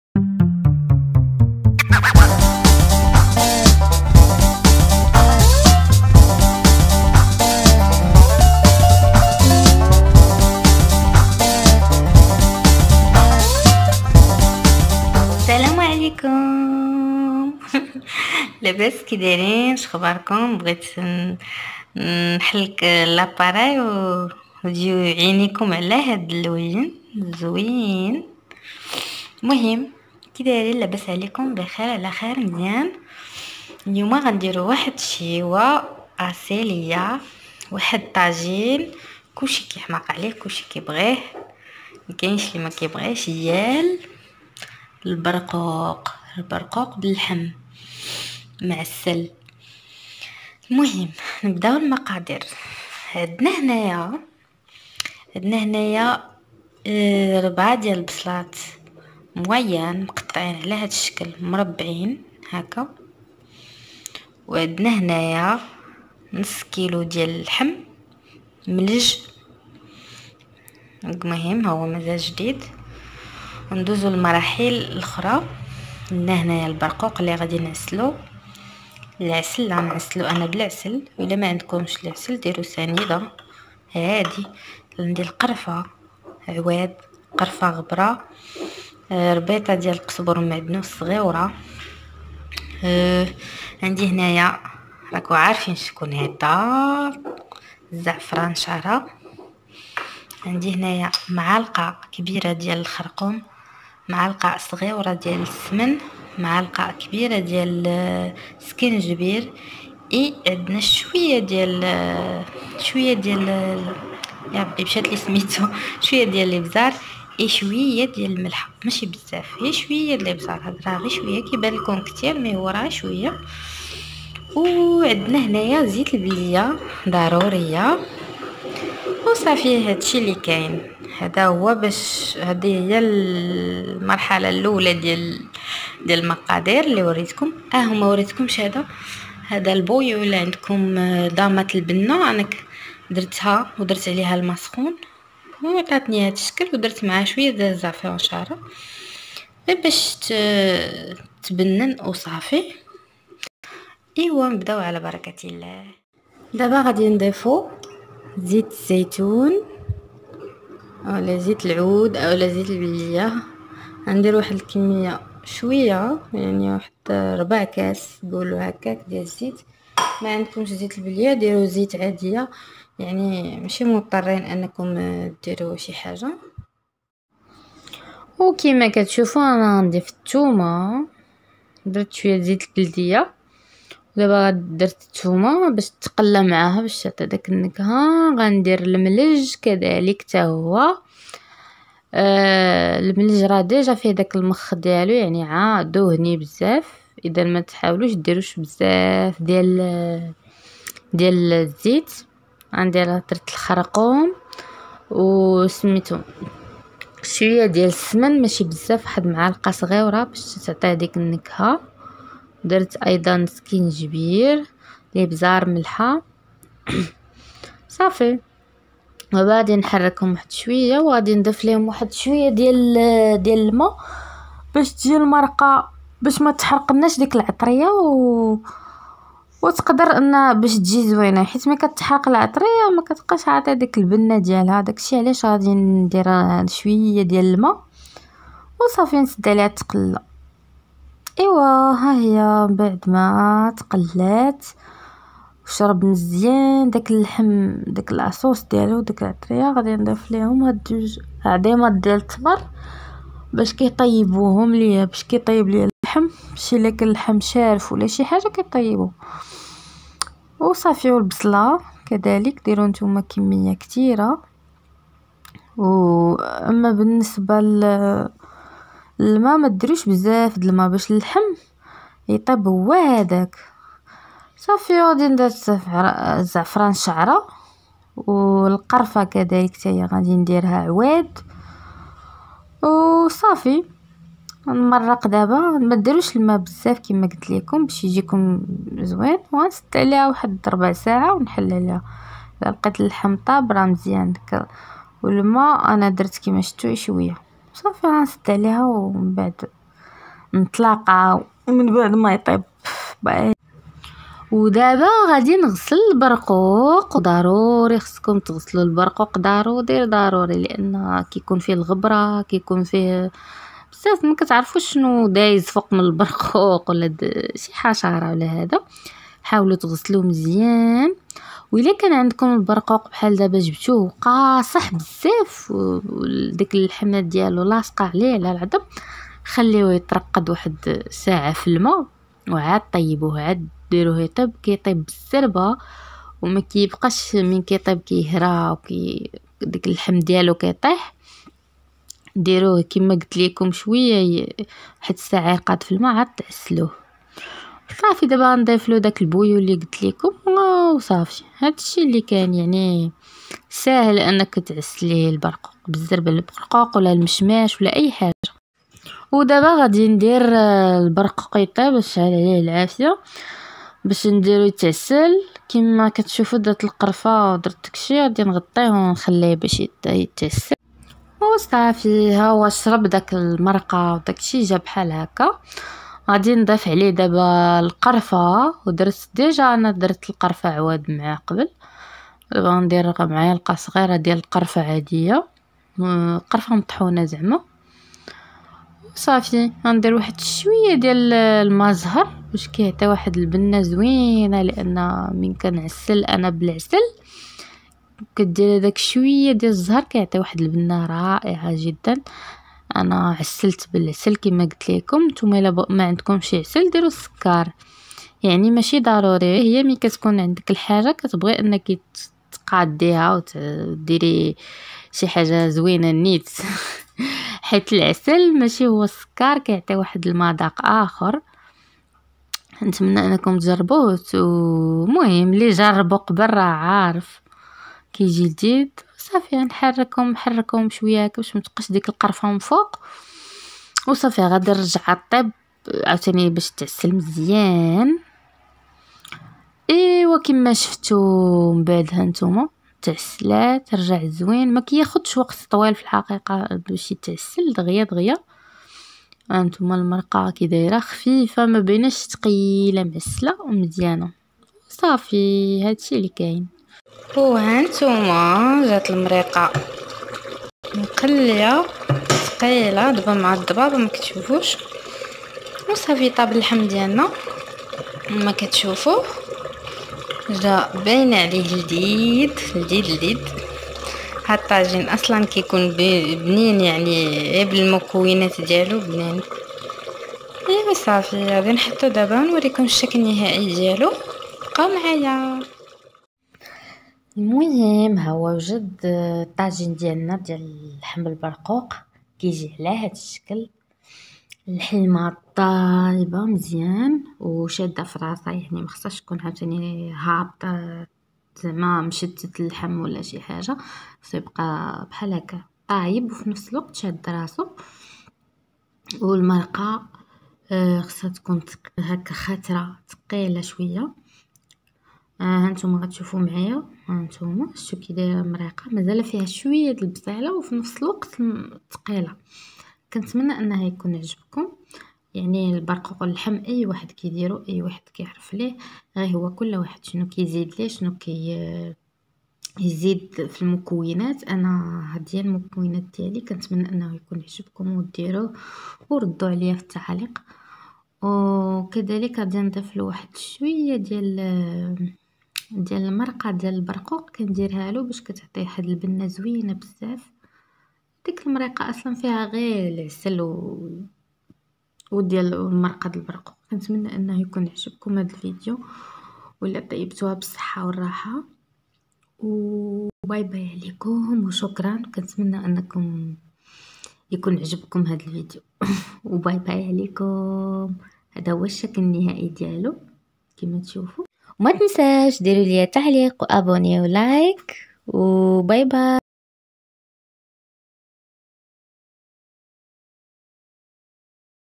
طاجين باللحم والبرقوق 🥩🥩🥩
طاجين جوج بصلات كبار كيلو لحم قرفه عواد قرفه غبره معلقه كبيره تسكن معالقه كبيره الخرقوم معلقه صغيره ديال الملحه معلقه صغيره ديال ليبزار اربعه معالق عسل قزبر معدنوس نصف كيلو برقوق مع القصه غيره اسمن درس من الثوم زعفران شعره زيت الزيتون